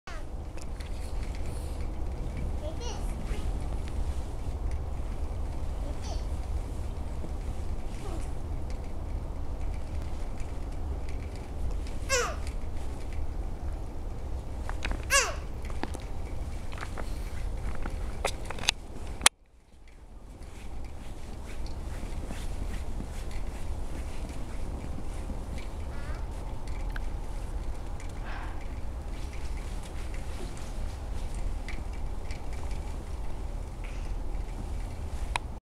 Vicious Snow Creatures
Early in the day, two strange snow creatures emerged from the wilderness.  This rare glimpse into their "dance of death" details how they camouflage their intentions to attack.  Known by many as the most ferocious killers on earth, this video shows how deceivingly cute the pair appears as they "dance" in an attempt to attract their next meal.